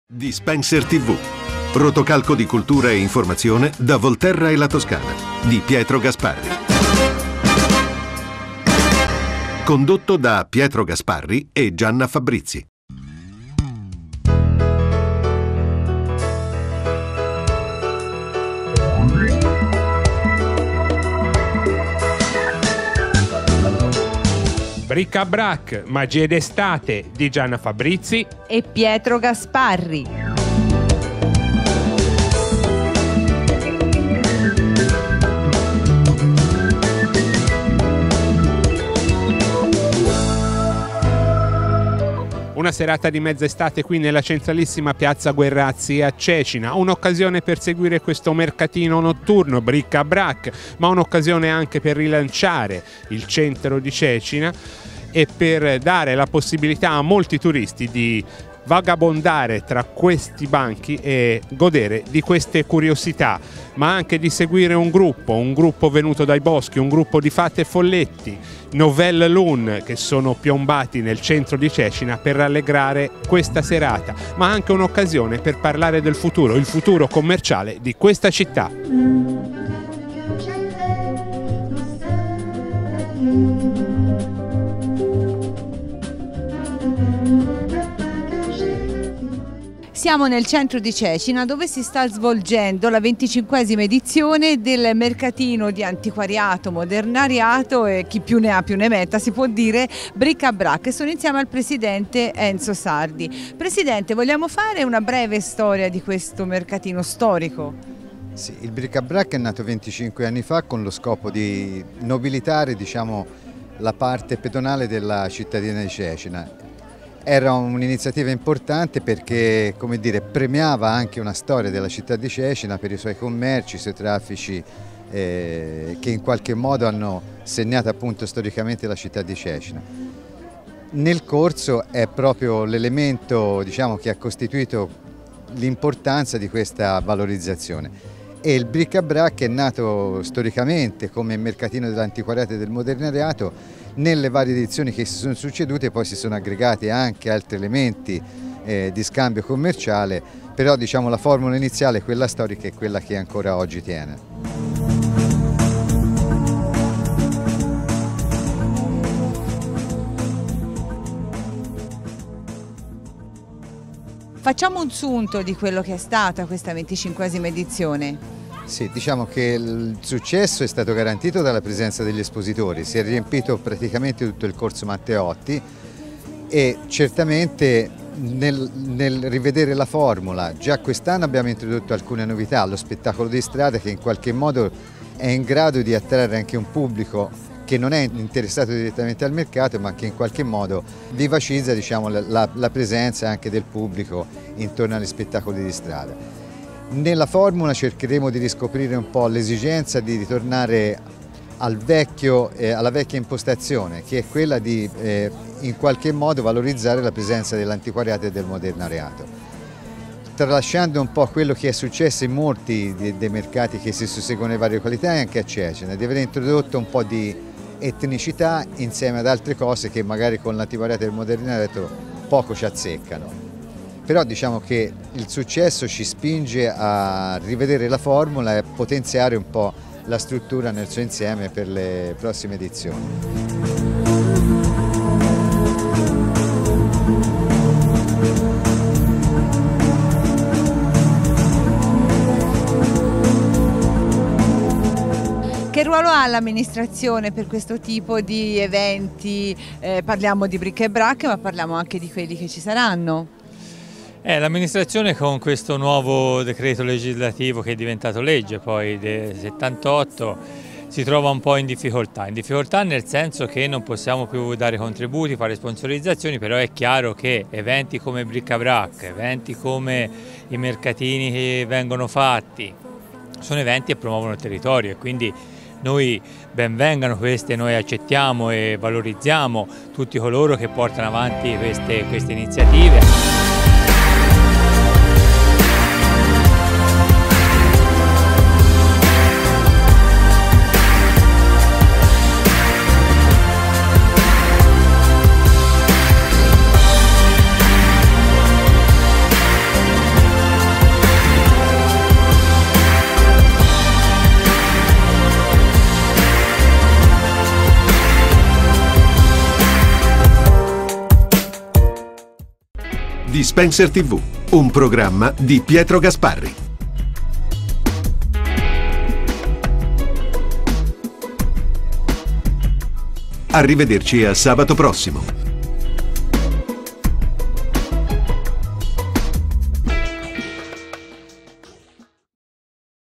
Bric-à-Brac il colori dell'estate
Anche se l'estate è ormai un ricordo, i colori della notte di Bric-à-Brac sono ancora vivi. Ed ecco che con la nostra "macchina del tempo" virtuale siamo nuovamente presenti in questo mercato coloratissimo di mezza estate a Cecina